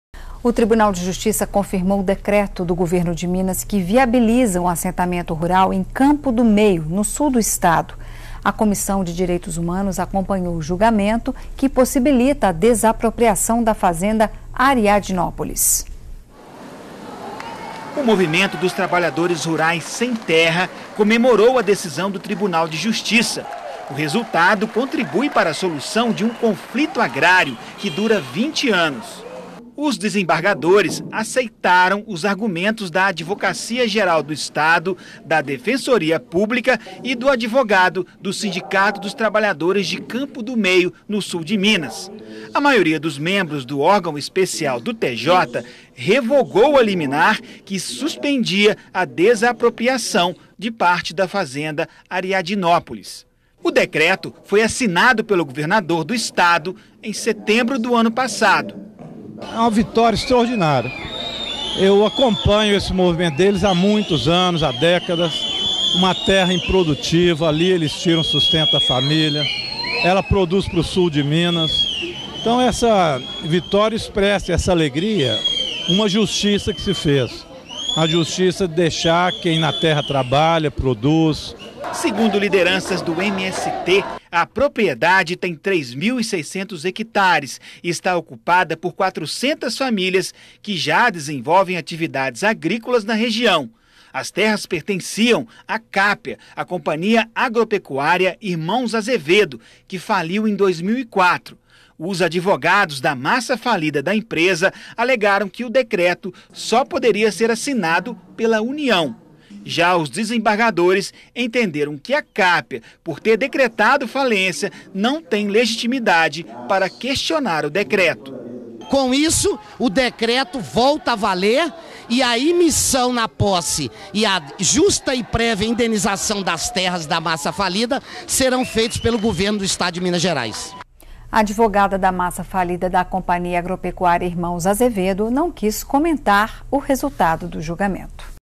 TJMG mantém decreto do Governador Pimentel que desapropria área da Fazenda Ariadnópolis
O Tribunal de Justiça confirmou o decreto do governo de Minas, que viabiliza um assentamento rural, em Campo do Meio, no Sul do estado. A Comissão de Direitos Humanos acompanhou o julgamento que viabiliza a desapropriação da Fazenda Ariadnópolis.